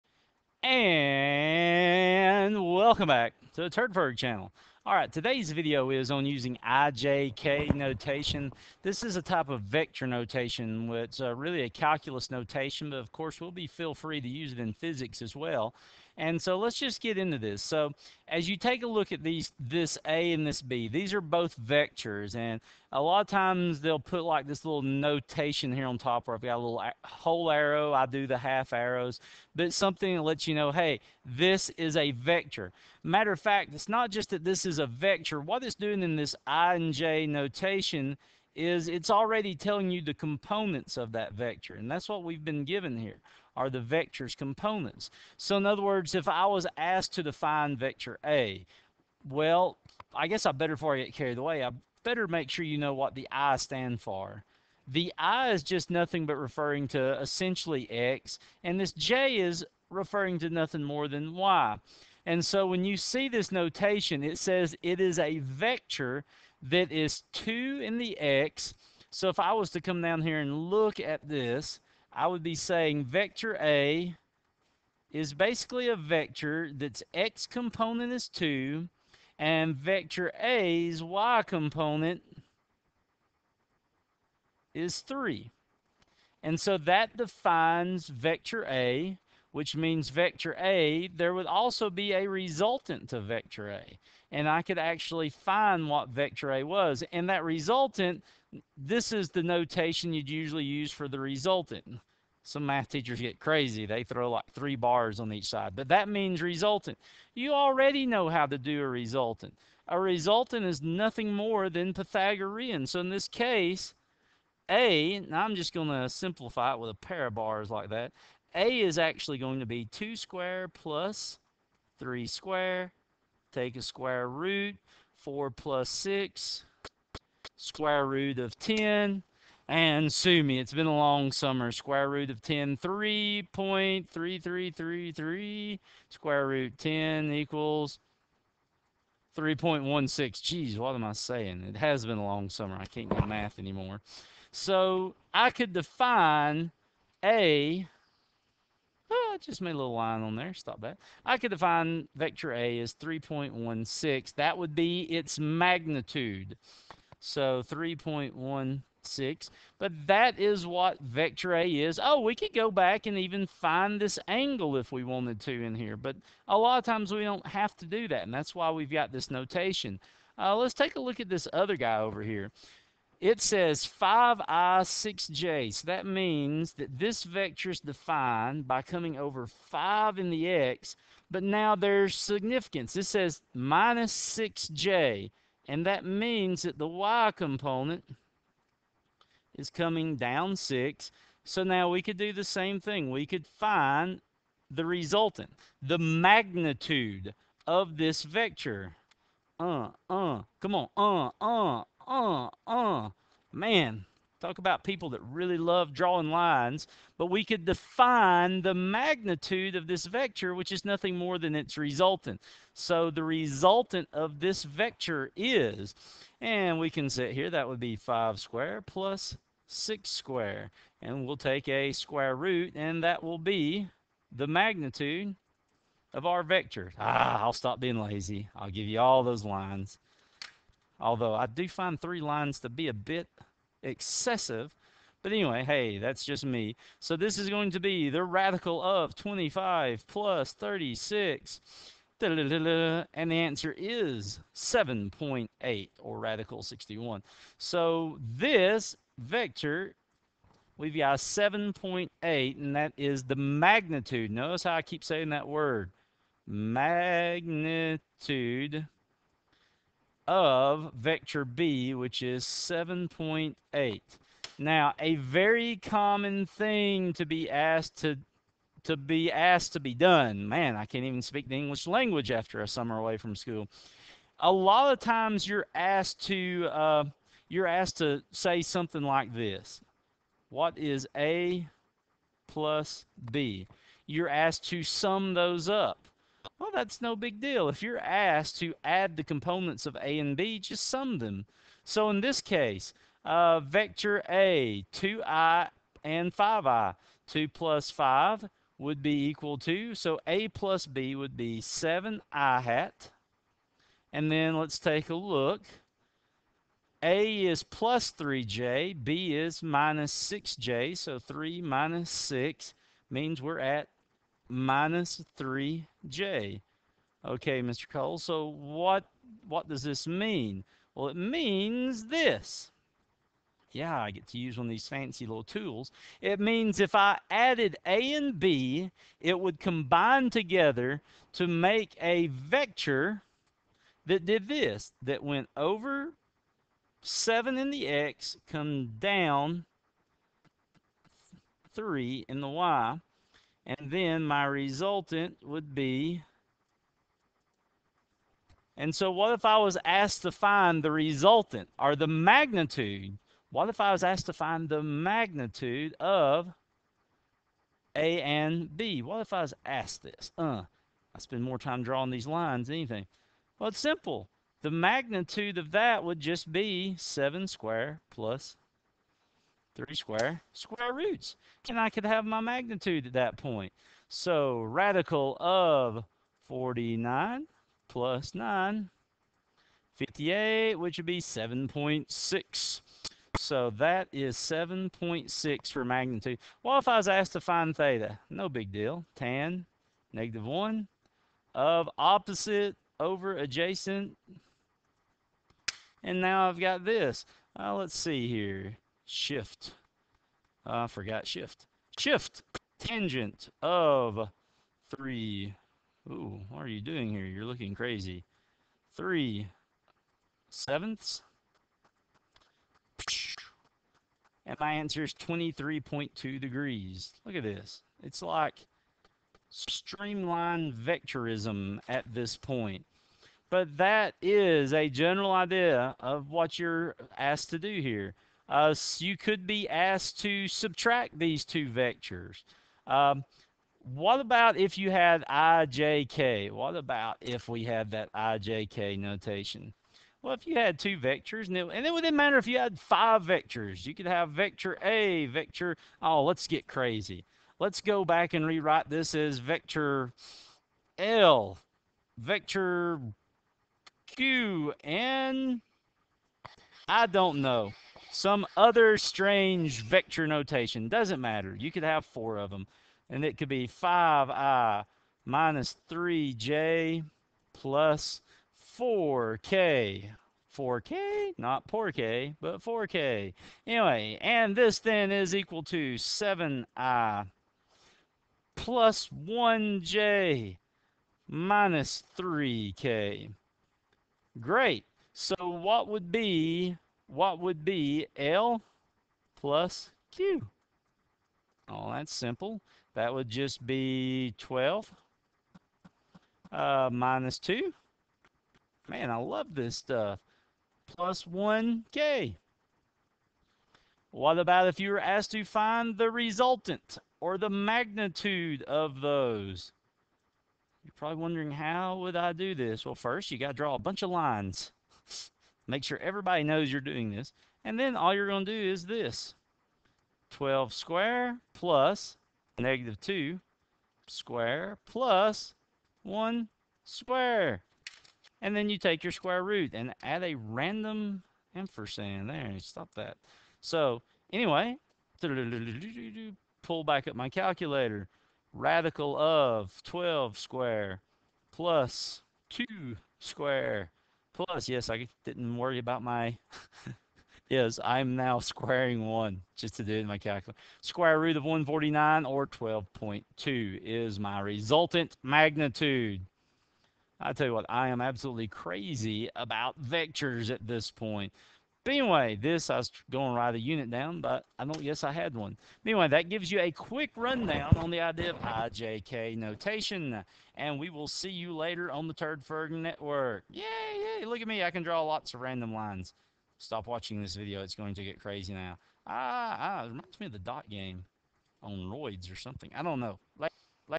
Physics - i j k vector notation introduction (MISTAKE at 2:06)
Using I j k notation in physics. @2:06 I had a brain malfunction and said 3 square is 6. When you make videos in the few sparring minutes between classes, well it happens. 5 preps, 7 classes in 7 periods, 3  Monster Energies a day for 17 years. My only two revelations are that I'm surprised I didn't mess up more, and that I never had a heart attack.